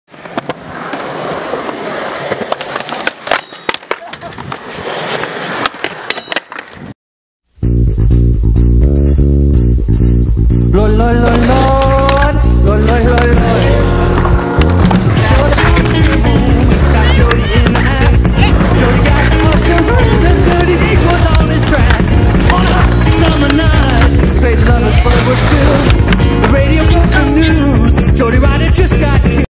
Frank's Cookout at Akron Skatepark 3
Another epic adventure. I got there late and didn't get to film much. Oh well.